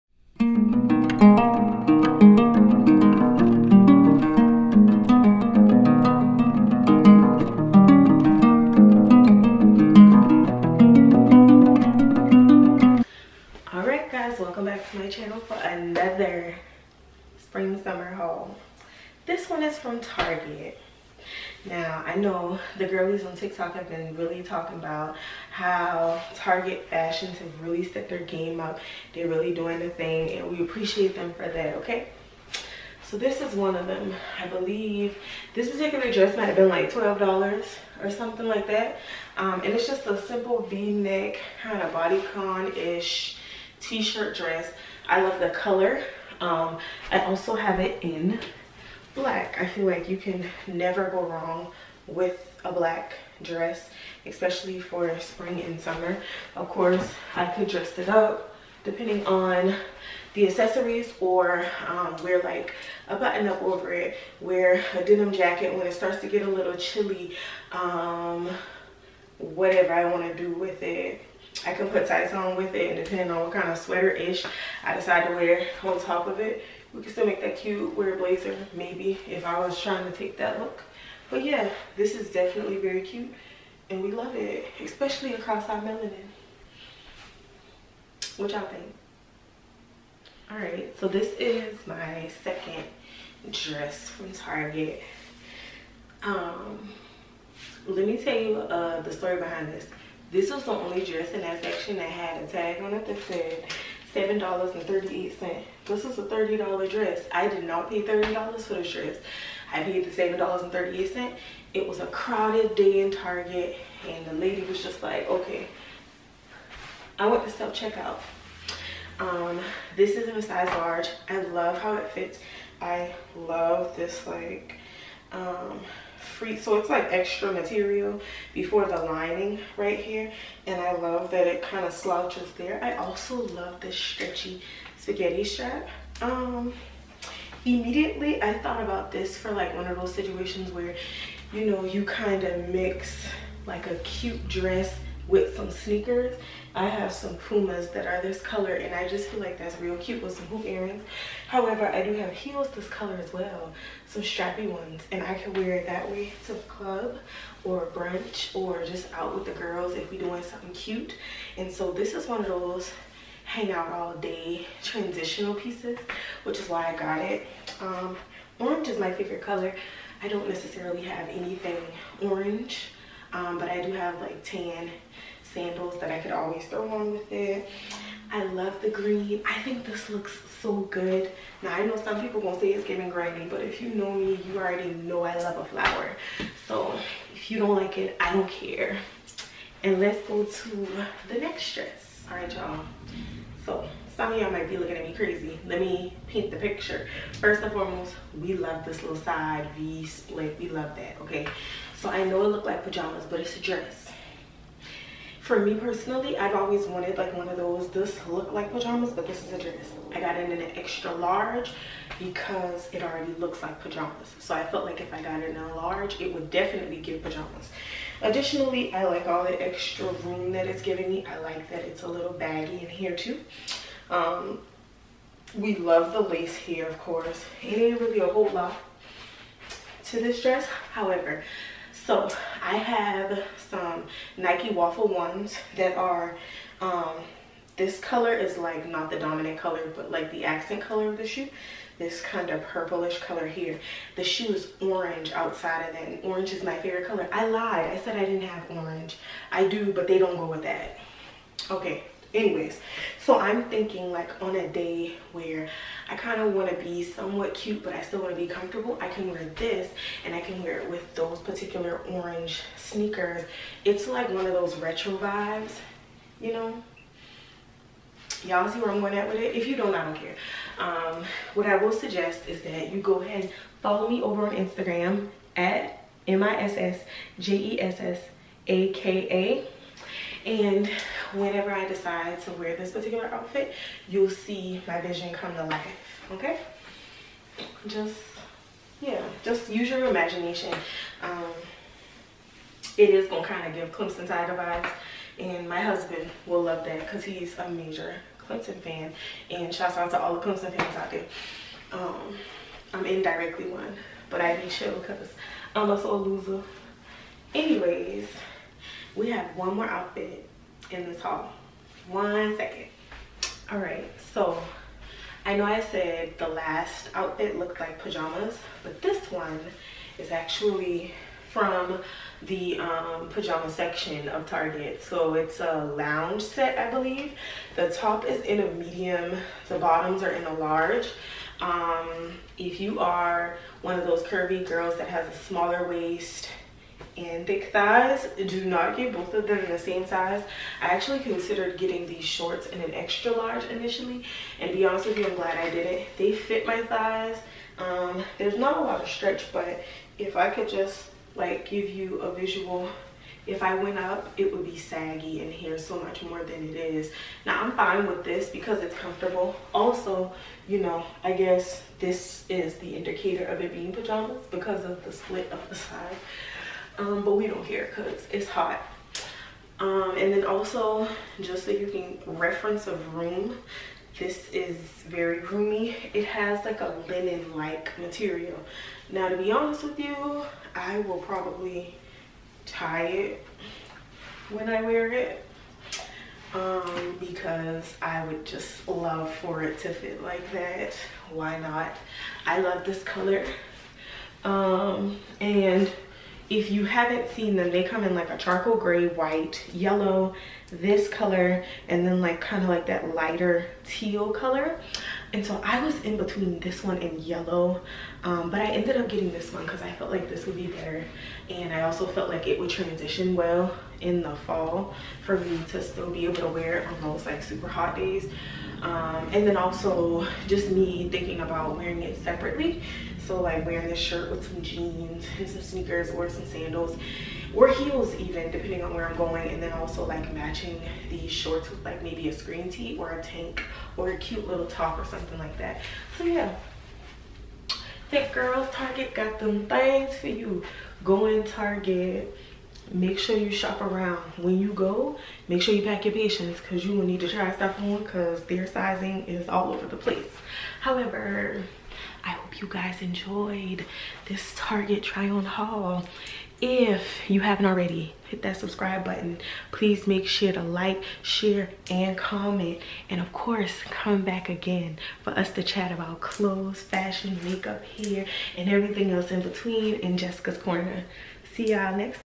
Target Fashion Finds Try On Haul
Enjoy this Target try on haul of a few fashion finds at a reasonable price.